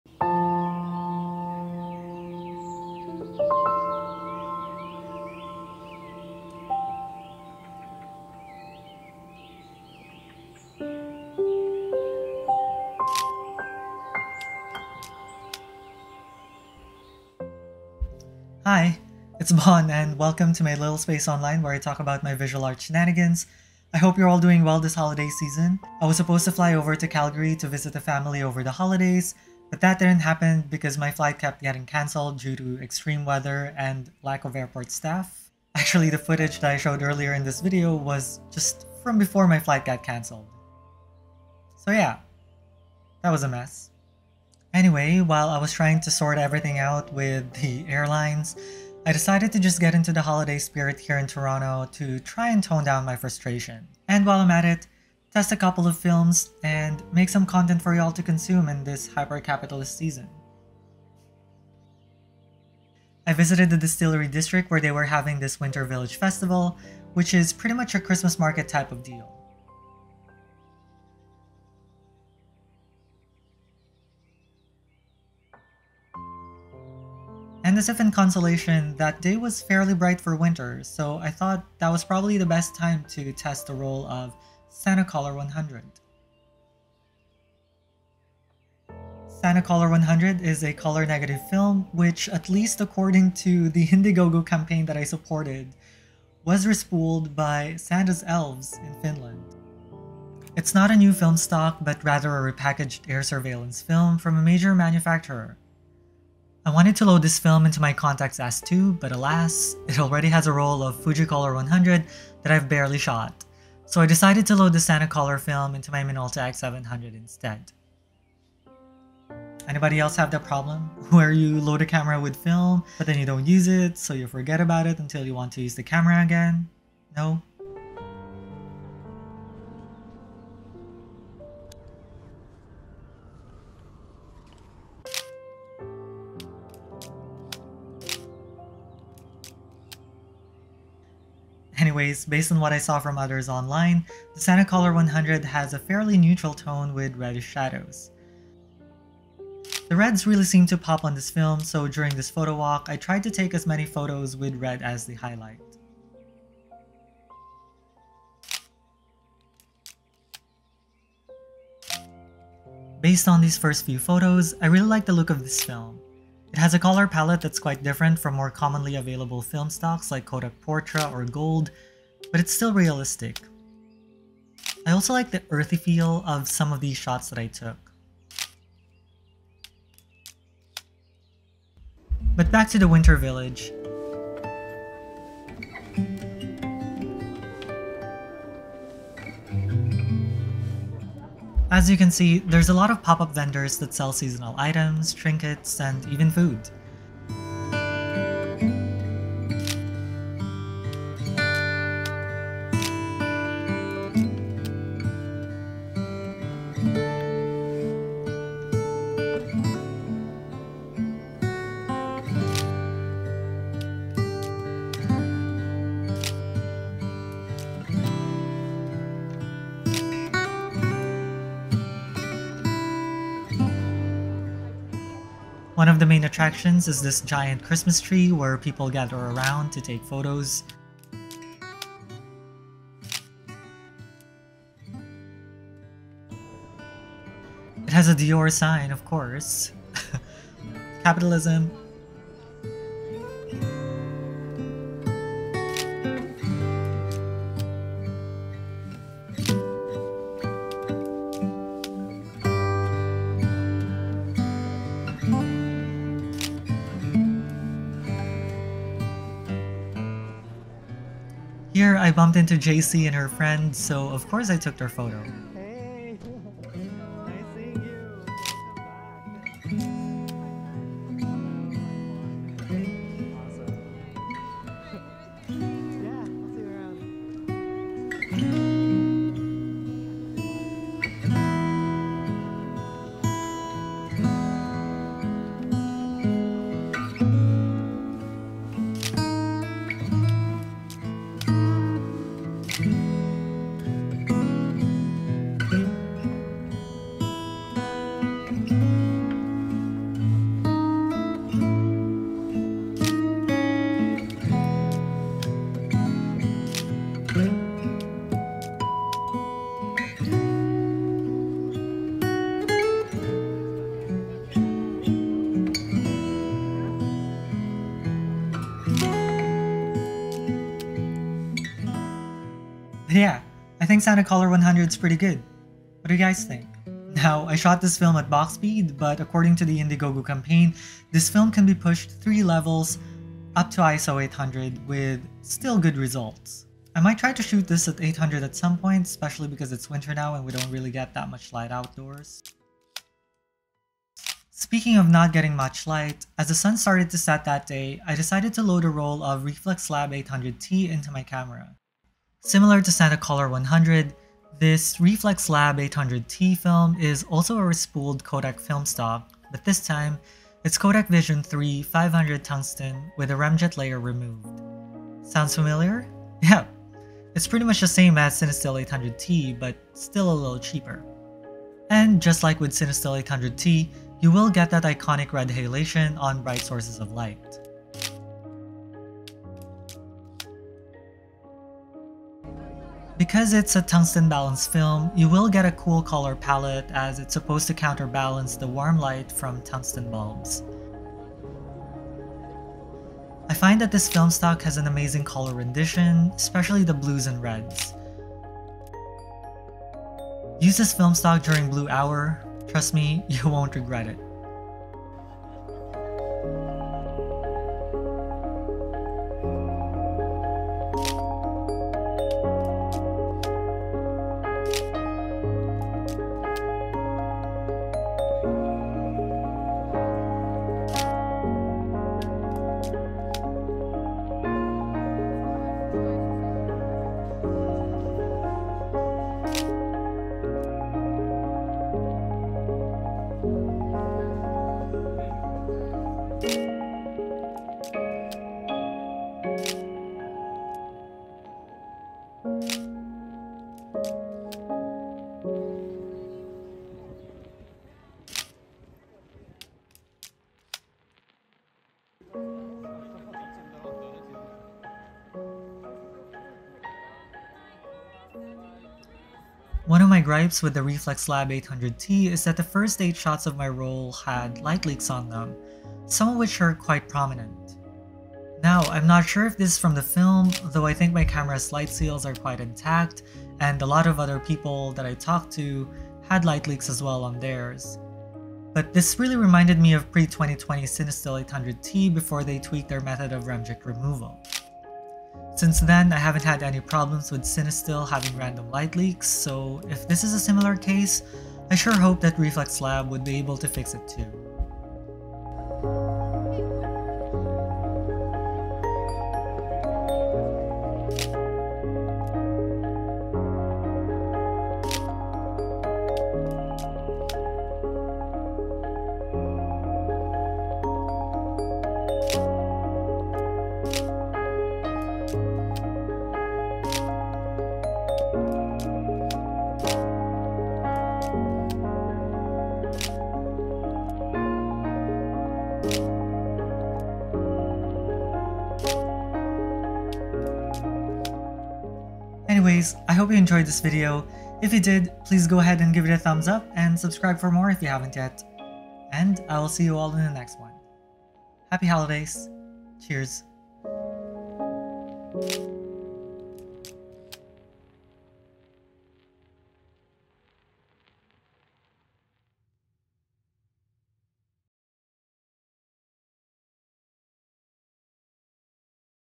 Christmas on SantaColor 100 and Reflx Lab 800T
I couldn't travel to visit my family this holiday season so, in this video, I show the results of my first time using a roll of SantaColor 100 and Reflx Lab 800T at Toronto's Distillery District Winter Village.

Happy Holidays! May the new year be good to us haha

▽ Chapters
00:00 Intro
01:25 SantaColor 100
05:46 Reflx Lab 800T